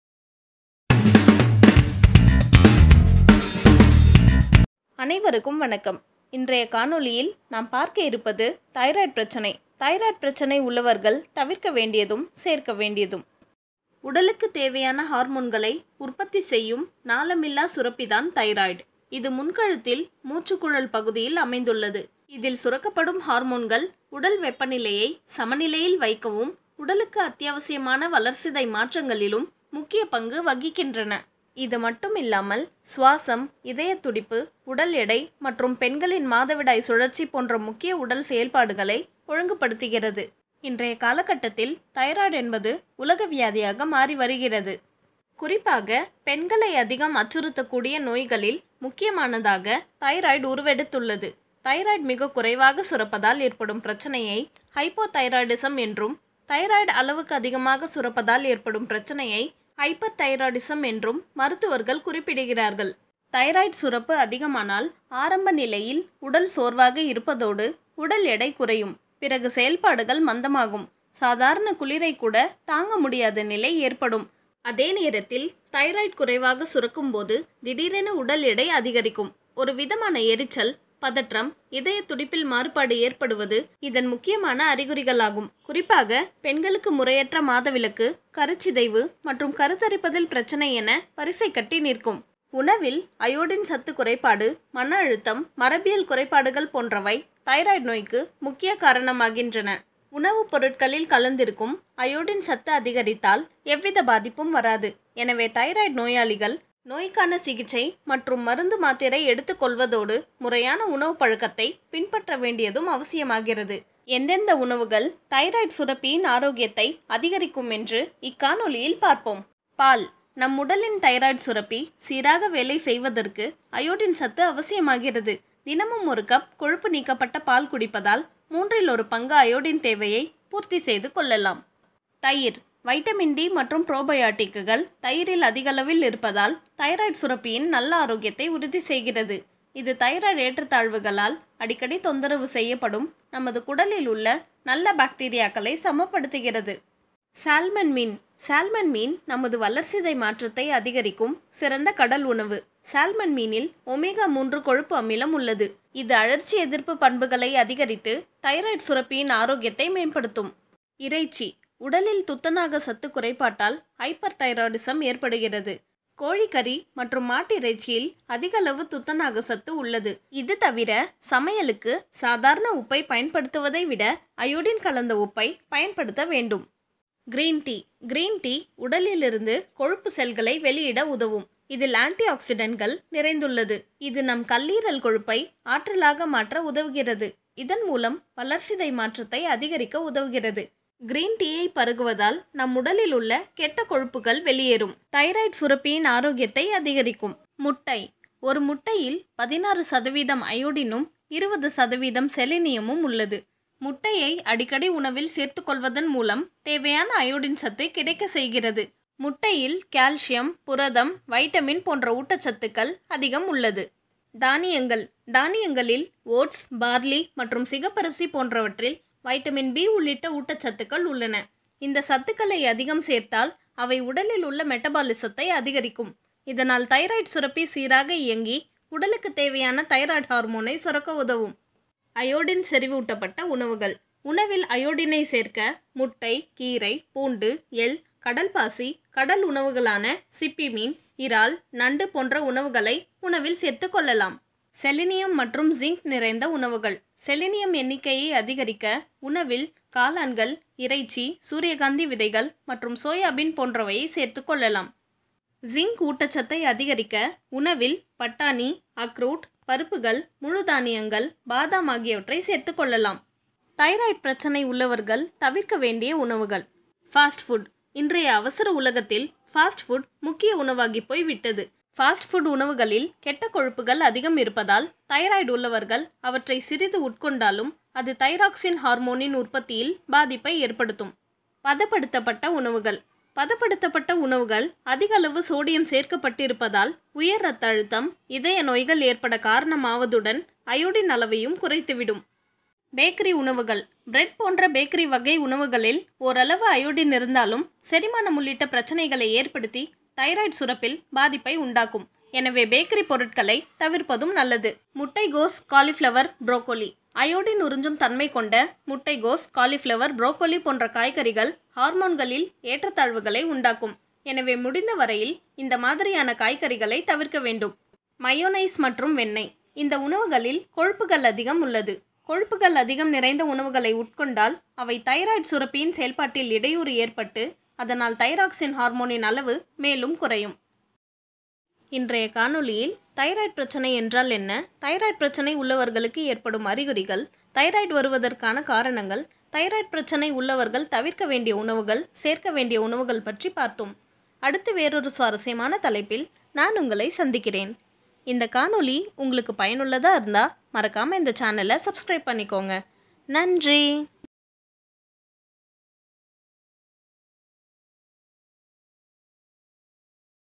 தைராய்டு என்றால் என்ன, தைராய்டு உள்ளவர்கள் தவிர்க்க வேண்டியதும் சேர்க்க வேண்டியதும்|Thyroid problem
தைராய்டு ஹார்மோனால் பாதிக்கப்படும் நான்கு முக்கிய உறுப்புகள்
4 Major Organs affected by Thyroid Hormone